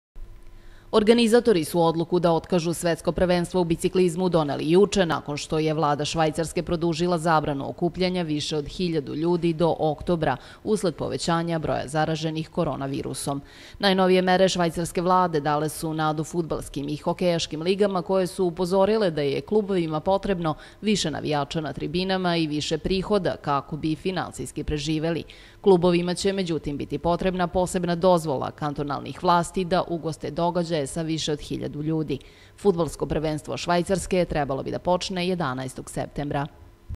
TV KANAL 9, NOVI SAD: OTKAZANO SP U BICIKLIZMU
Zbog pandemije kovida 19 otkazano je i Svetsko prvenstvo u biciklizmu, koje je trebalo da bude održano od 20. do 27. septembra u Švajcarskoj.